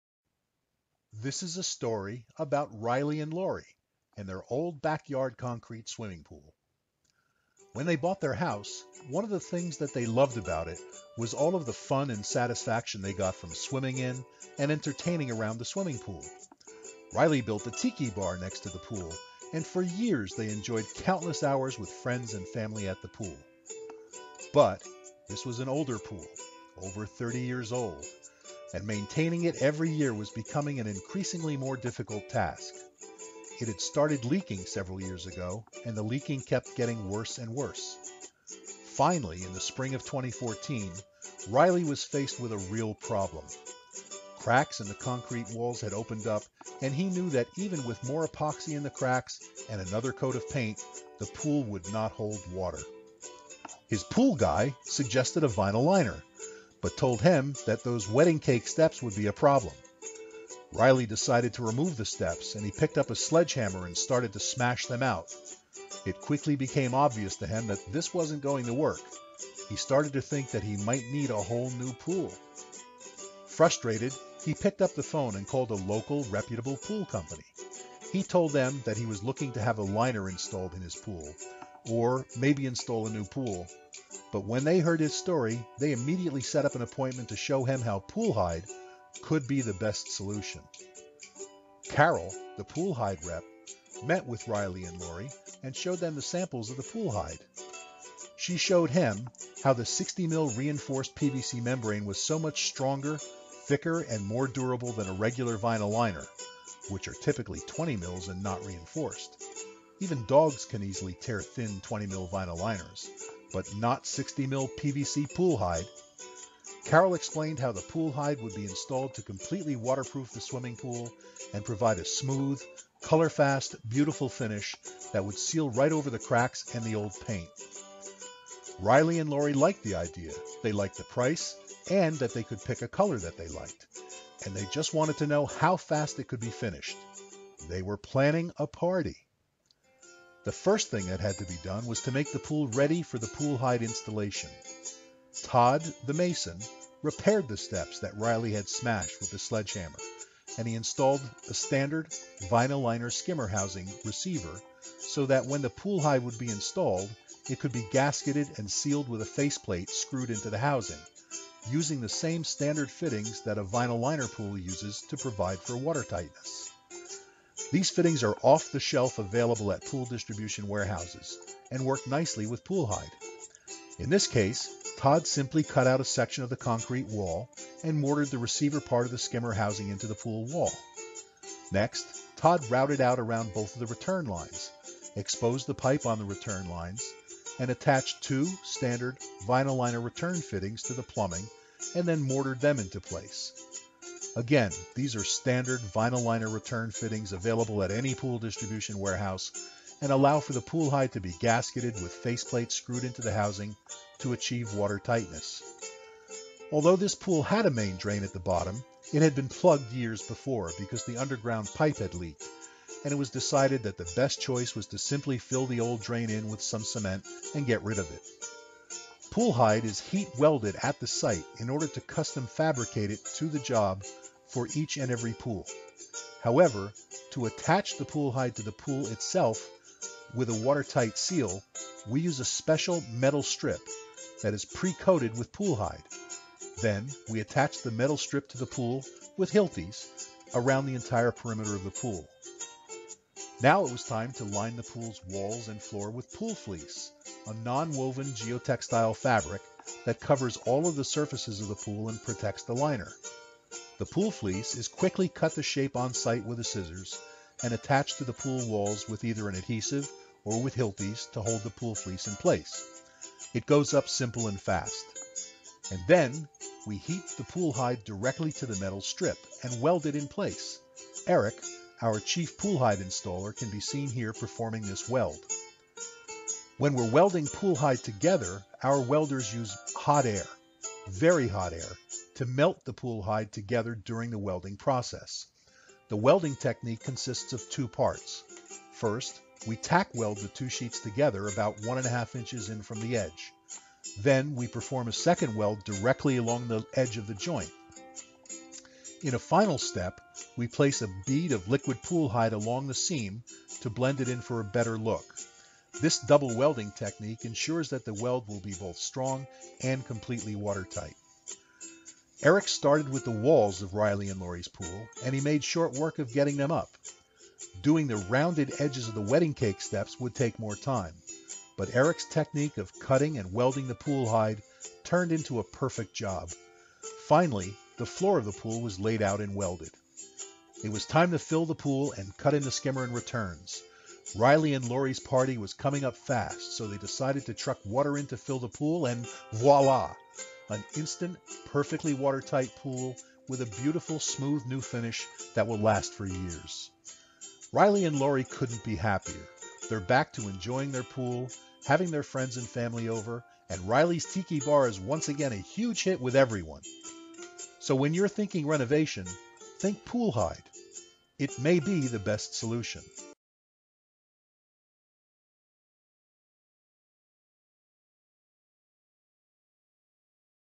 PoolHide® - Riley and Laurie Swimming Pool Renovation
Do you have an old concrete or vinyl liner swimming pool which is cracked or leaking? Renovating with PoolHide® 60 Mil Reinforced PVC Membrane from Park Pool and Pond Products may be the right solution.